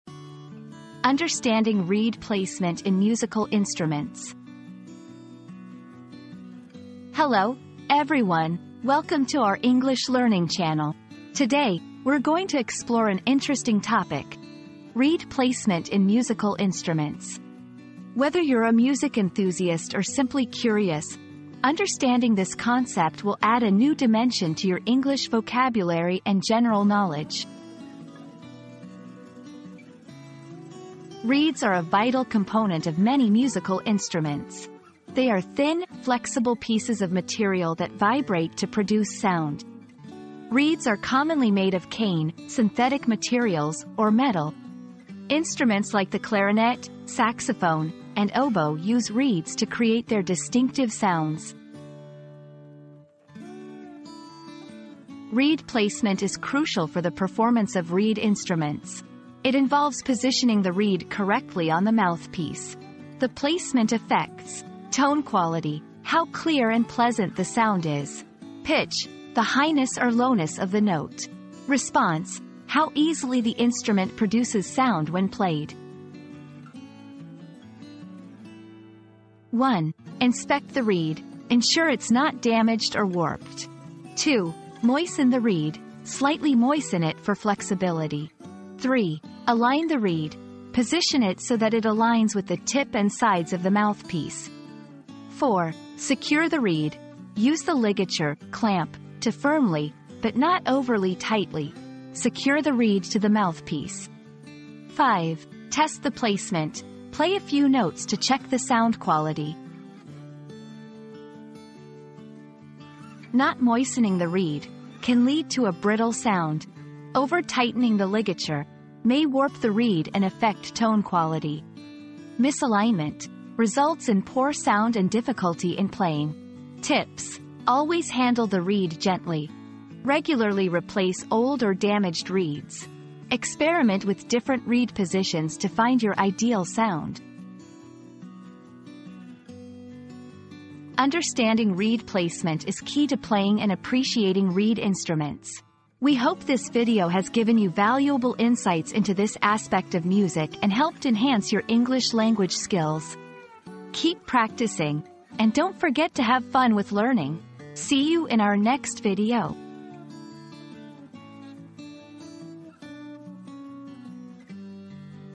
Understanding Reed Placement in Musical Instruments
Unlocking the Melodic Mysteries: Understanding Reed Placement in Musical Instruments • Discover the secrets behind the captivating melodies as we delve into the intricate world of reed placement in musical instruments. From clarinets to saxophones, this video unravels the science and artistry behind achieving the perfect harmony through strategic reed positioning. Get ready to embark on a musical journey like no other!

00:00 • Introduction - Understanding Reed Placement in Musical Instruments
00:30 • What is a Reed?
00:54 • The Importance of Reed Placement
01:18 • Steps for Proper Reed Placement
01:52 • Common Mistakes and Tips